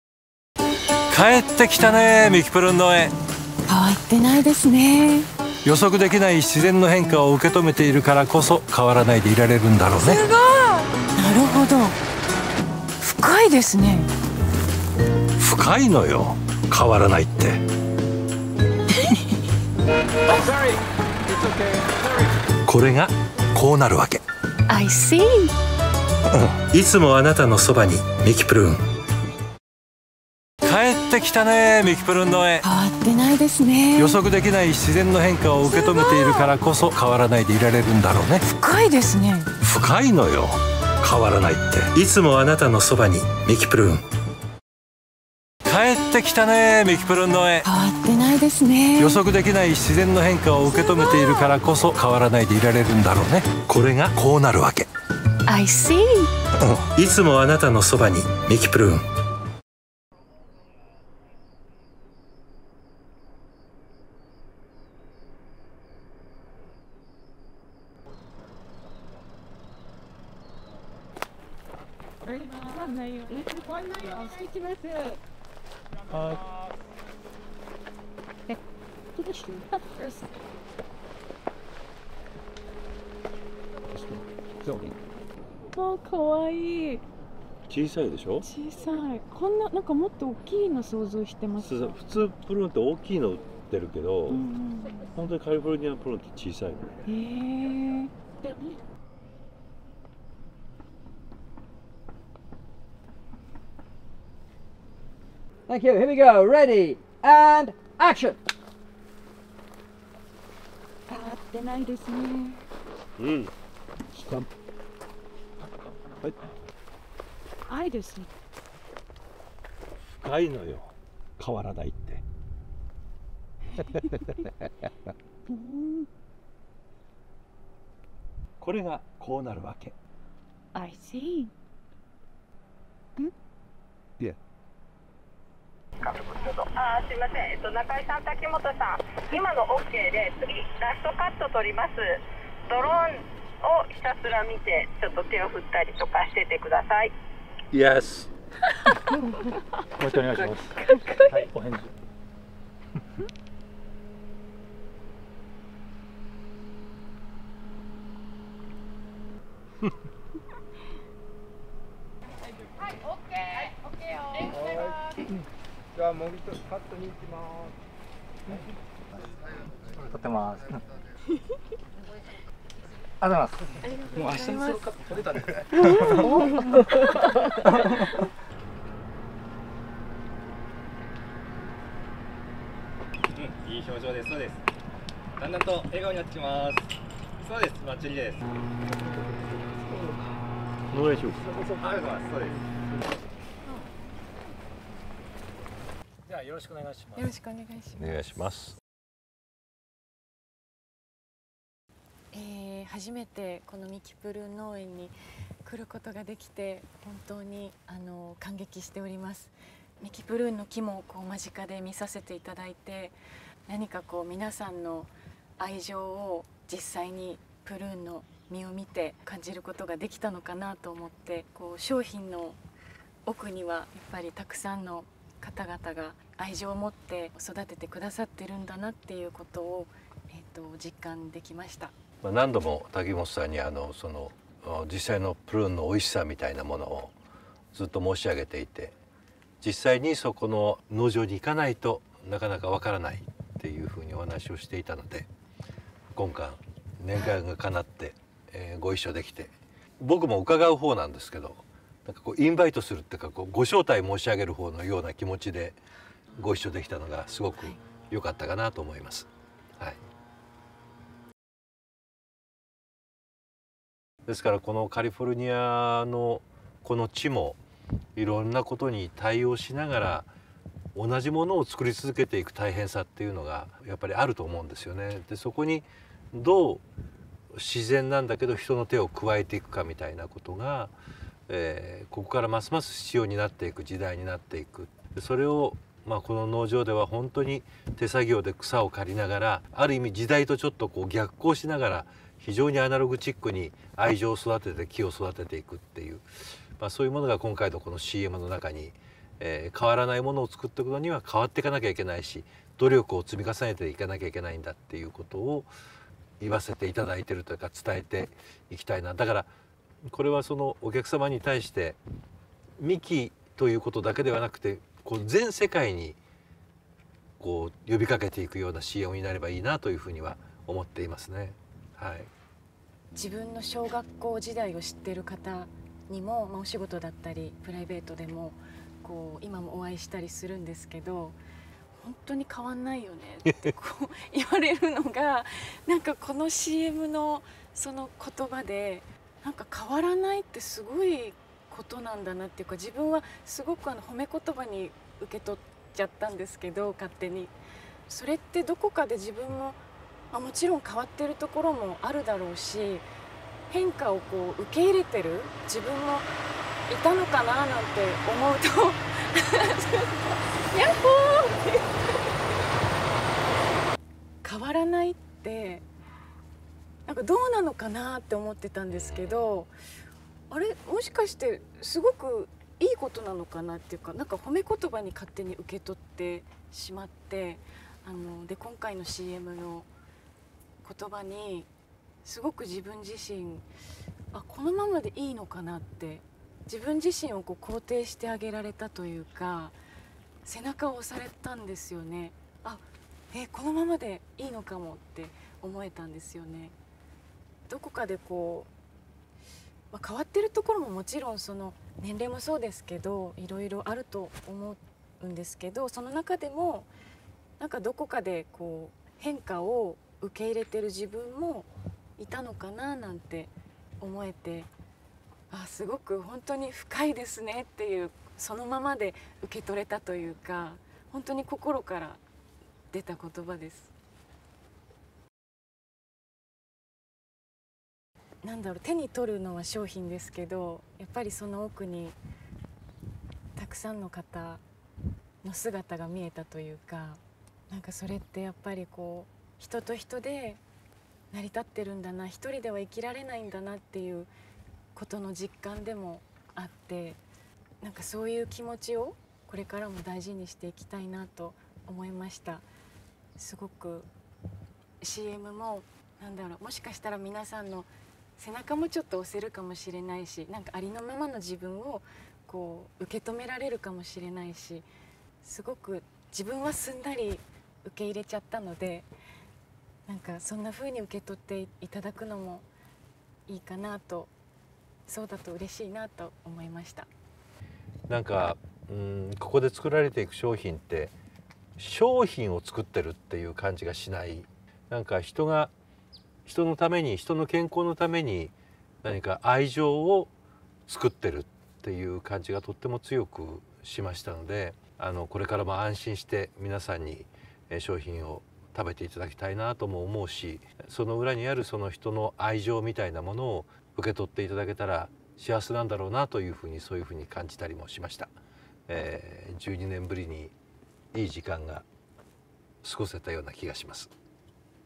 瀧本美織、中井貴一の突然の英語に爆笑！インタビュー中にトラックが通るも元気に「ヤッホー！」 ミキプルーン新ＣＭ＆メイキング公開
俳優の中井貴一と瀧本美織が出演する、ミキプルーン新ＣＭ『帰ってきたミキプルーン農園』篇『変わらないって深い』篇とメイキング・インタビュー映像が公開された。